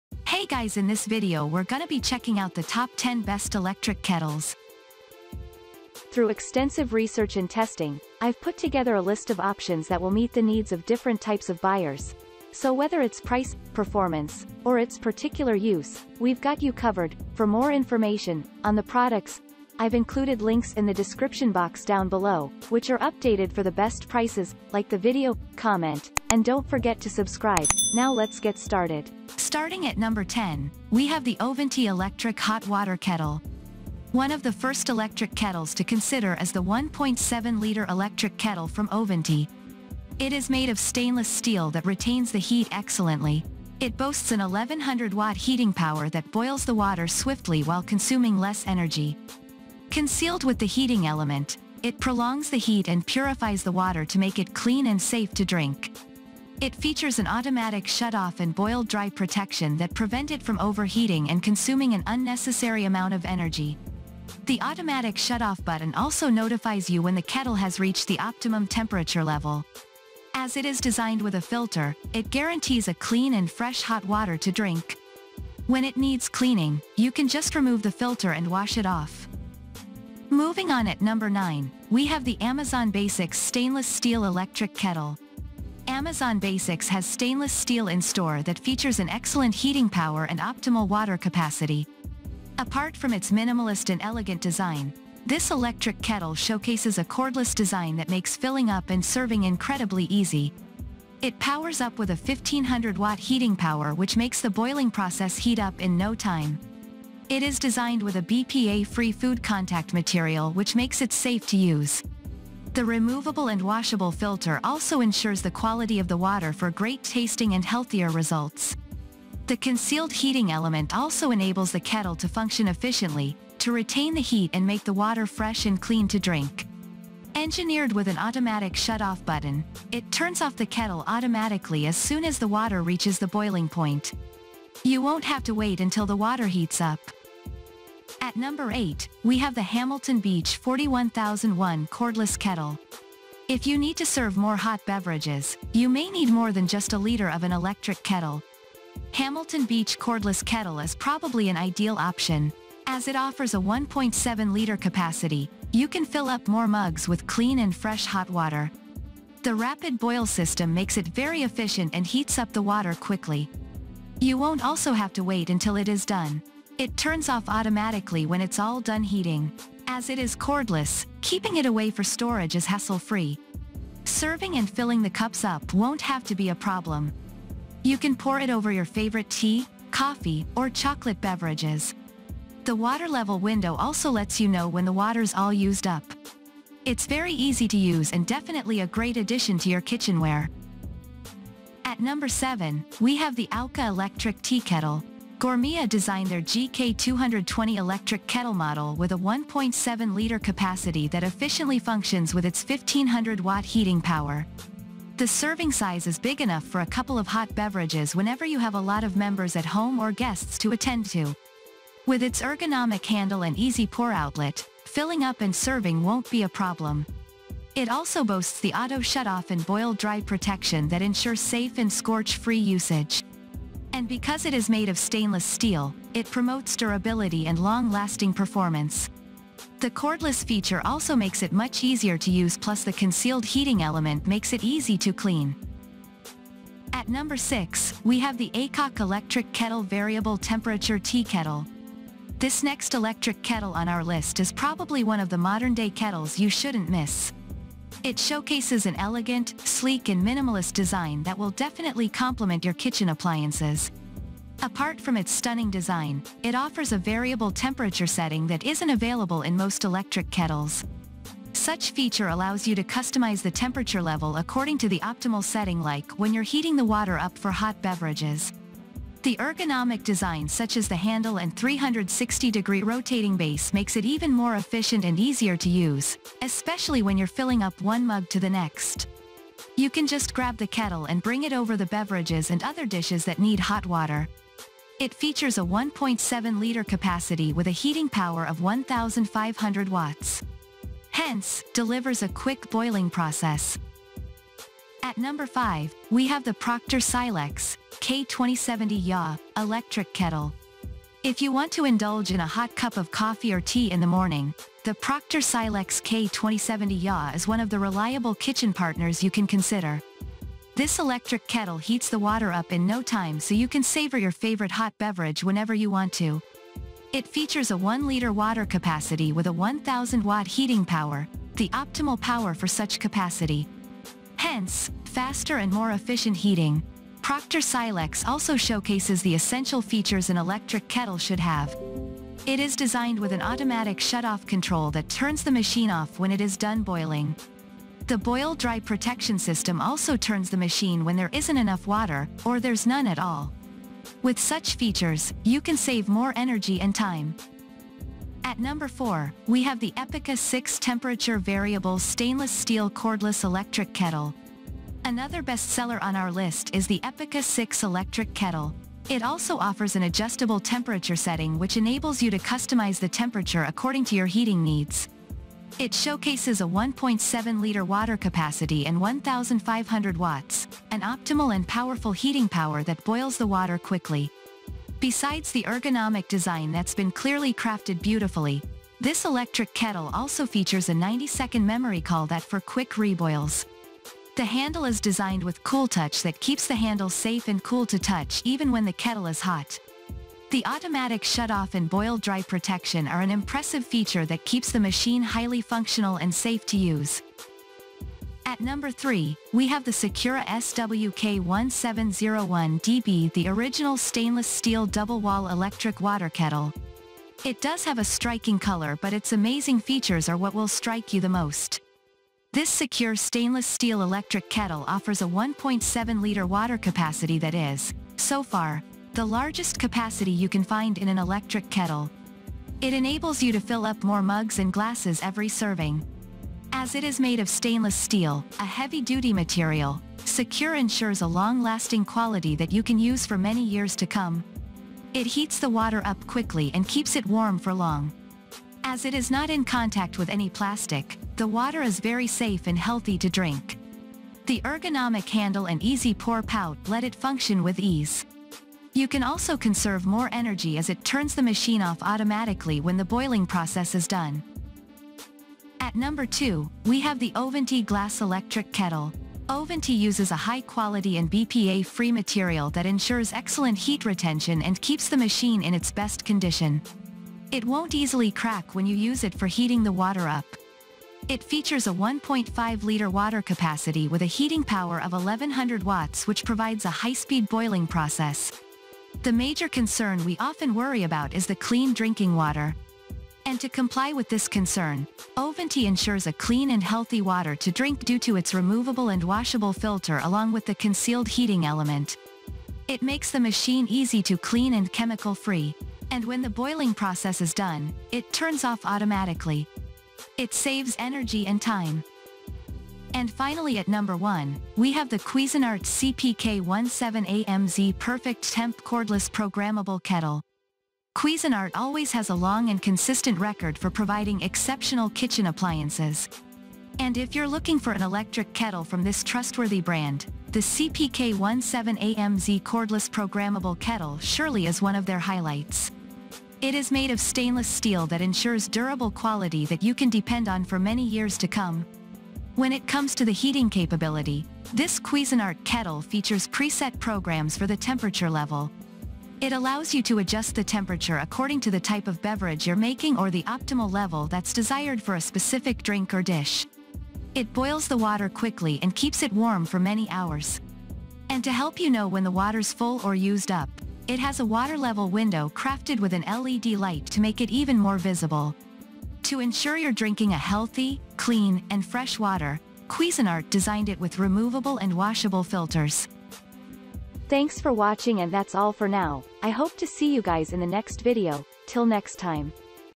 ✅Top 10 Best Electric Kettles of 2023
► Links to the Best Electric Kettles you saw in this video

✅10. Ovente Electric Hot Water Kettle
✅9. AmazonBasics Stainless Steel Electric Kettle
✅8. Hamilton Beach 41001 Cordless Kettle
✅7. Auka Electric Tea Kettle
✅6. Aicok Electric Kettle Variable Temperature Tea Kettle
✅5. Proctor Silex (K2070YA) Electric Kettle
✅4. Epica 6-Temperature Variable Stainless Steel Cordless Electric Kettle
✅3. Secura SWK-1701DB The Original Stainless Steel Double Wall Electric Water Kettle
✅2. Ovente Glass Electric Kettle
✅1. Cuisinart CPK-17AMZ Perfect Temp Cordless Programmable Kettle

To save you both time and money, we’ve narrowed down to some of the best Electric Kettles.
Check out an in-depth review of the best Electric Kettles in 2023.

In this video, we make a new research on the top best Electric Kettles.
This is the best Electric Kettles review.
Best time for buying your new Electric Kettles.